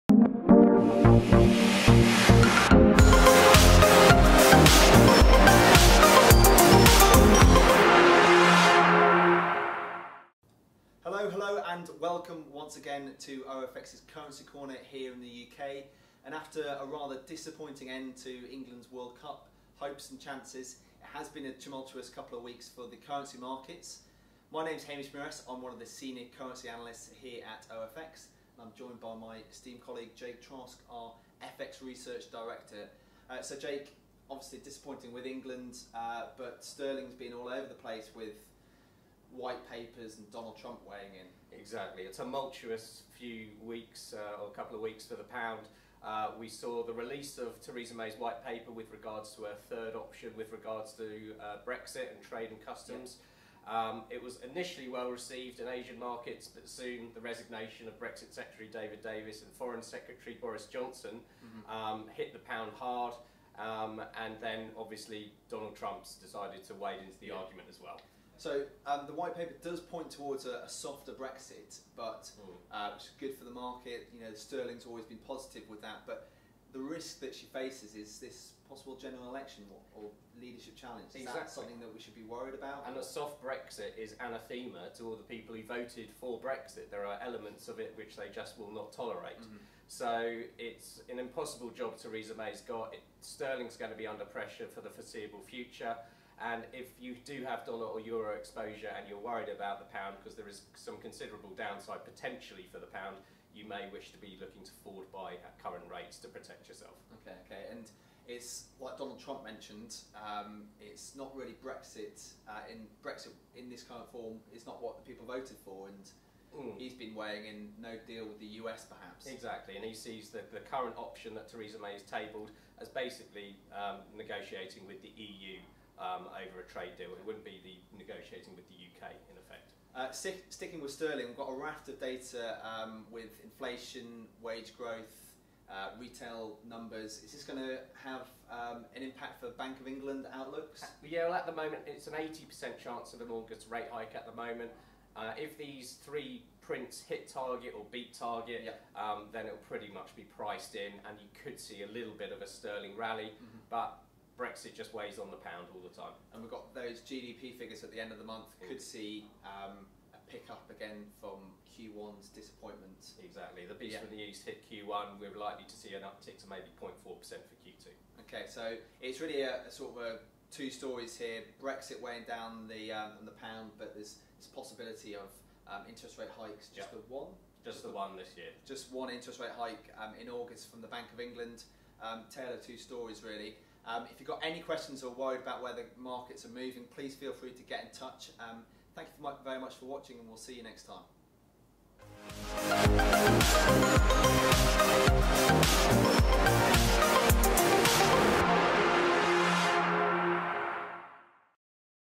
OFX Currency Corner (UK & Europe) 17 July 2018
It's a Tale of Two Cities here in the UK. On one hand, the BoE is most likely implementing a rate hike in August, which would normally bolster sterling.  But all things Brexit continue to heavily weigh it down. With Boris' and other key resignations seemingly coming in droves, it's a good time for UK businesses to get on the front foot with their currency strategy to batten down the hatches whilst more Brexit drama unfolds in the next few months.